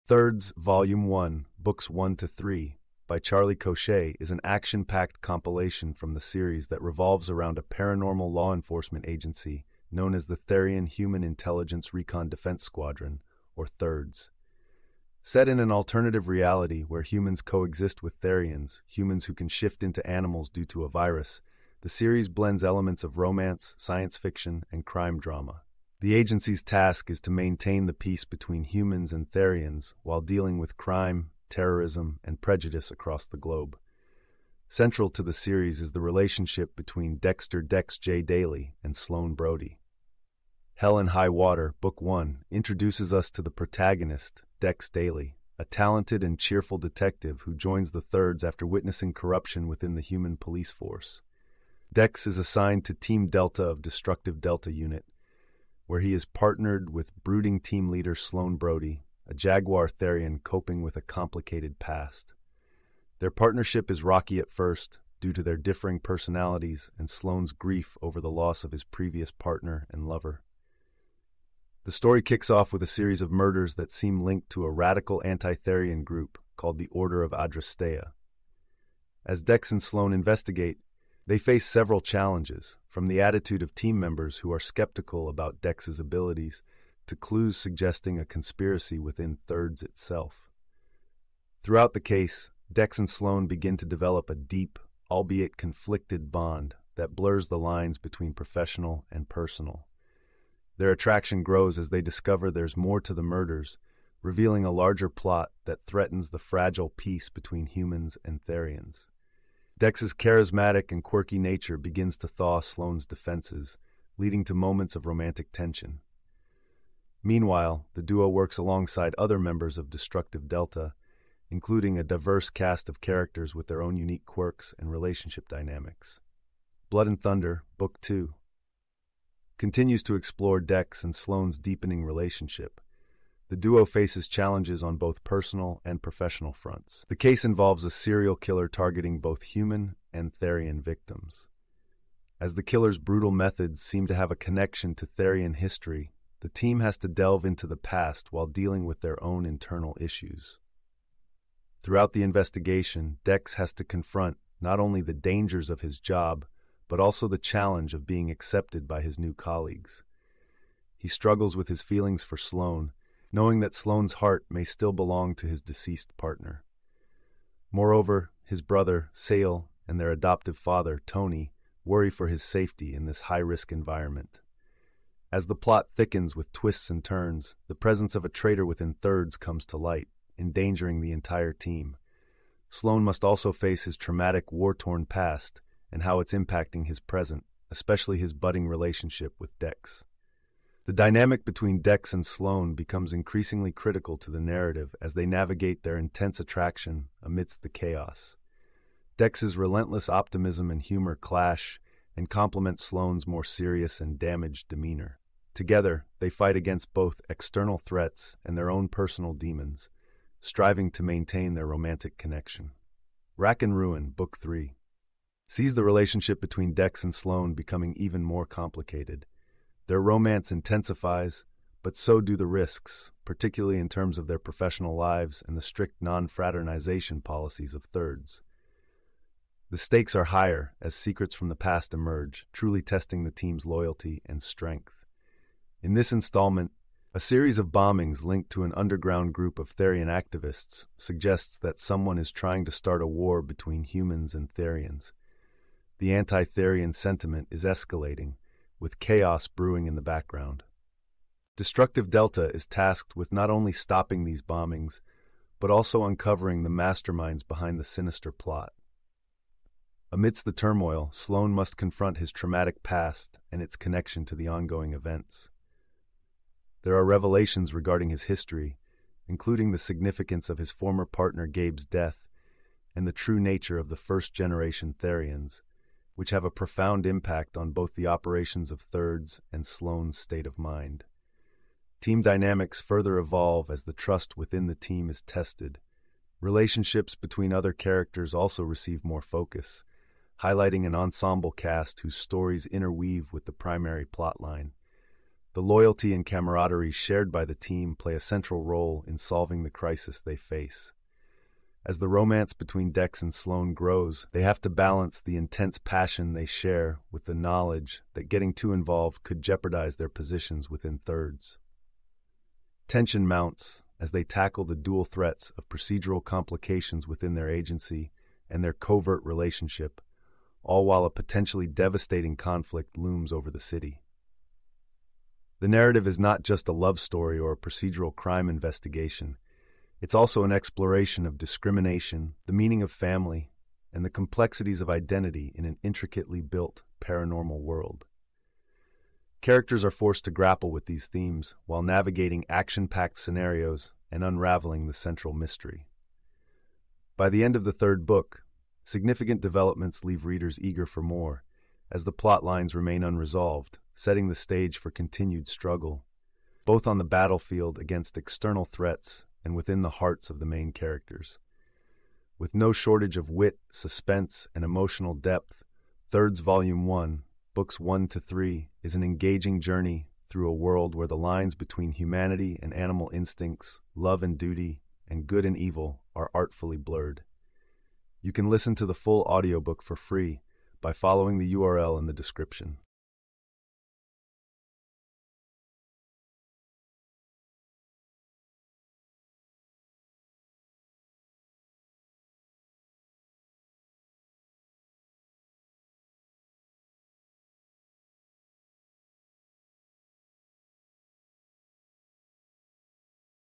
THIRDS Volume One: Books 1-3: The THIRDS Collection, Book 1
Get the Full Audiobook for Free:
"THIRDS Volume One: Books 1-3" is a collection of the first three installments in Charlie Cochet's paranormal romance series, following a team of gay shape-shifting agents as they navigate crime-solving, conspiracies, and complex relationships within a law enforcement agency designed to keep peace between humans and Therians (shifters).